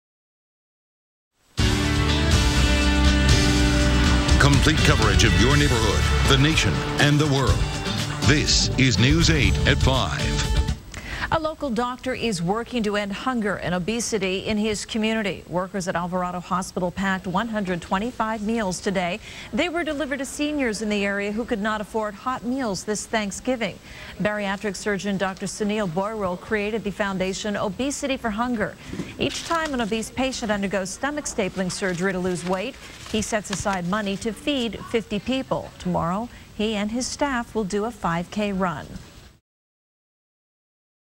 Obesity for Hunger - Bariatric Surgeon Eradicating Hunger in San Diego - Alvarado Hospital
With an ambitious goal to eradicate hunger in San Diego, Dr. Sunil Bhoyrul, a bariatric surgeon, donates part of his surgery fee to feed 50 people. He also is working with local companies to participate iin his foundation "Obesity for Hunger." Dr. Bhoyrul teamed with Alvarado Hospital staff to feed more than 100 homebound seniors on Thanksgiving.